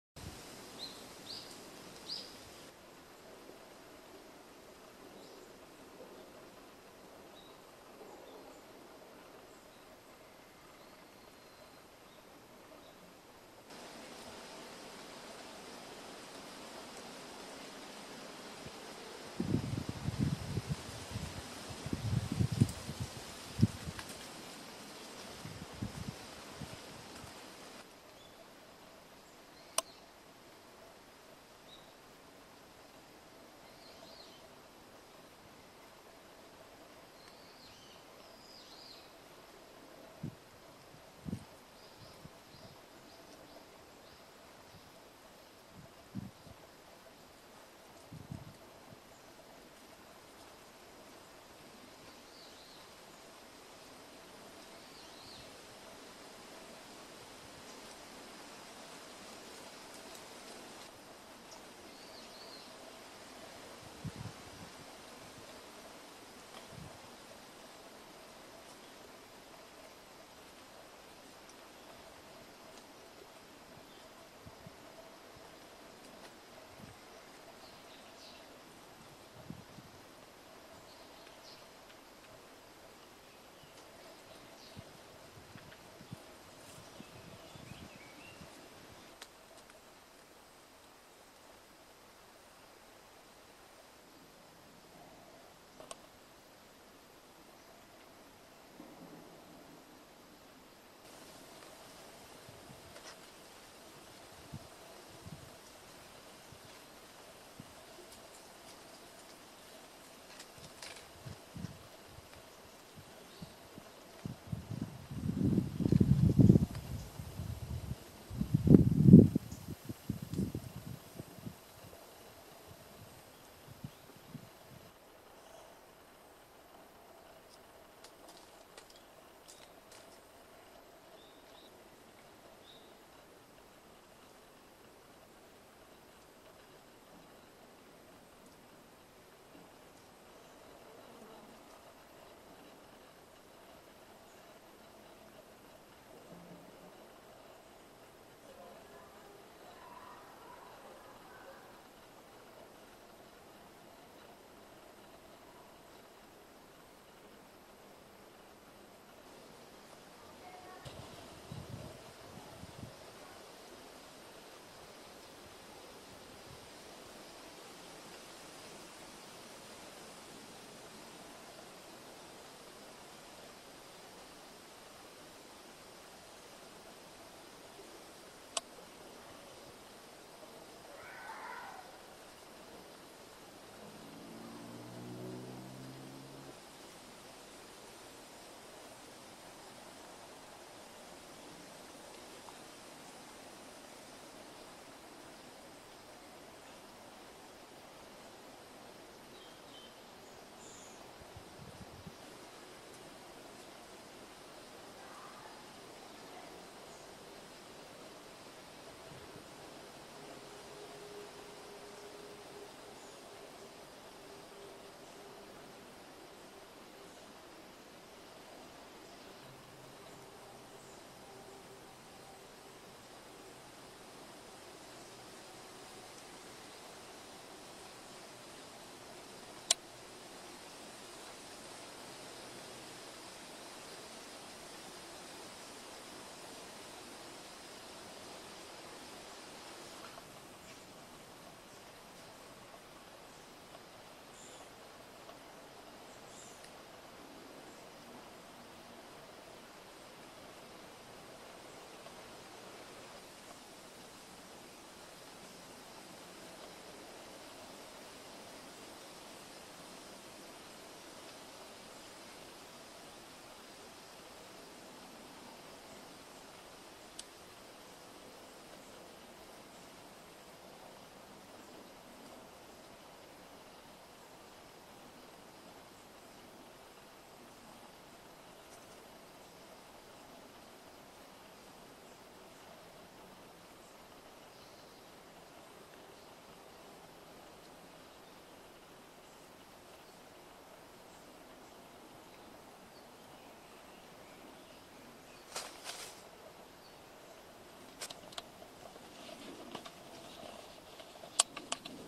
陽だまりの落葉上に出てきたニホンカナヘビ Takydromus tachydromoides
2019年11月30日午後、山口市徳地町の山間部で撮影しました。晩秋の雑木林は、落ち葉で覆われていました。曇りがちでしたが、やっと雲が切れて日が差してきました。陽だまりの落葉上にカナヘビを見つけました。